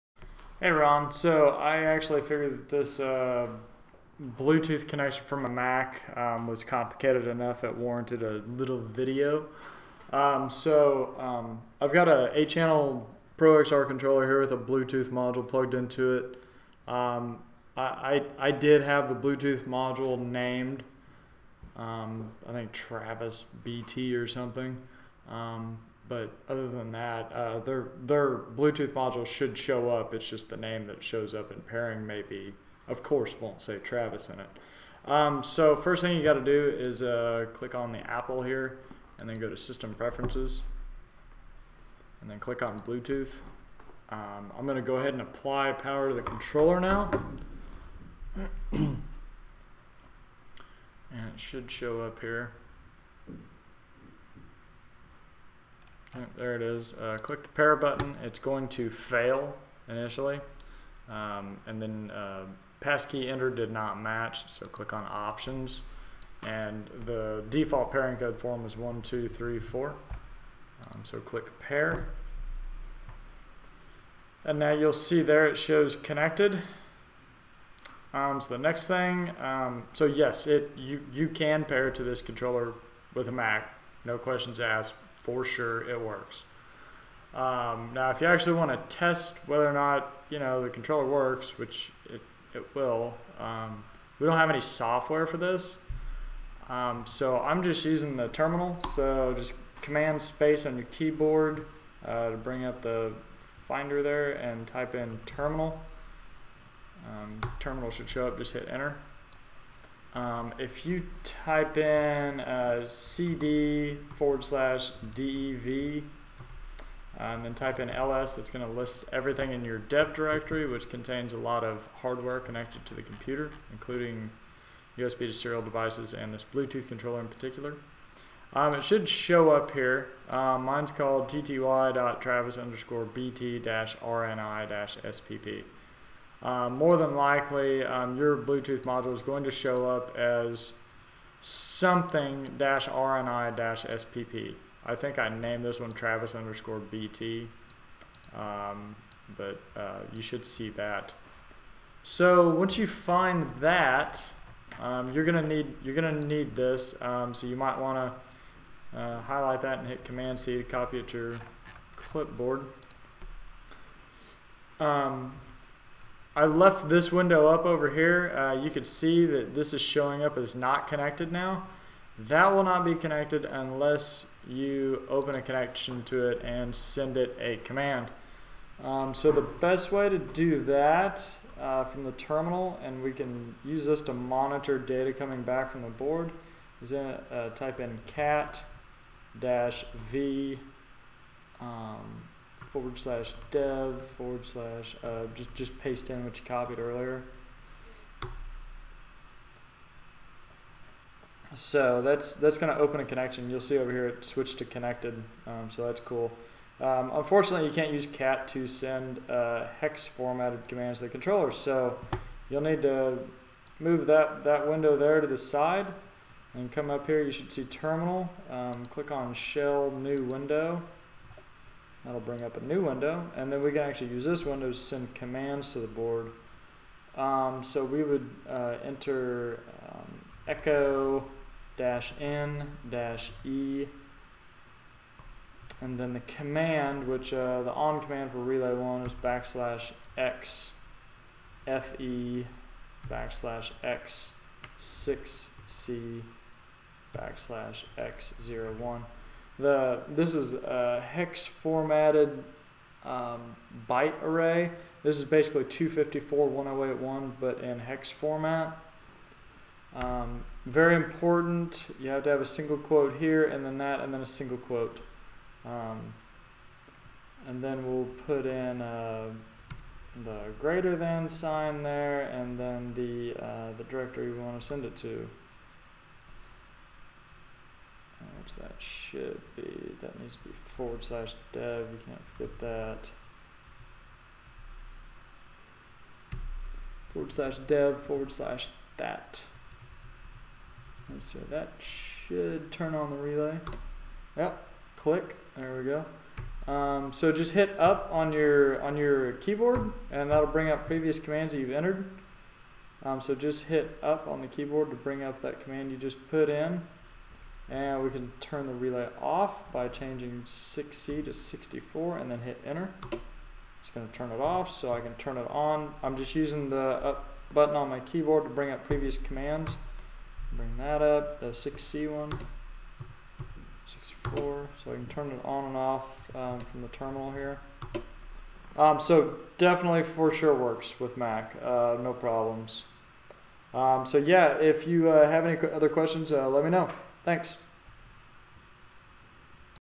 Bluetooth Mac Terminal
Communicating to an NCD Bluetooth relay on a Mac is possible, although it can be a bit tricky.  This shows the steps to establish Bluetooth communication and controlling the relay on a Mac.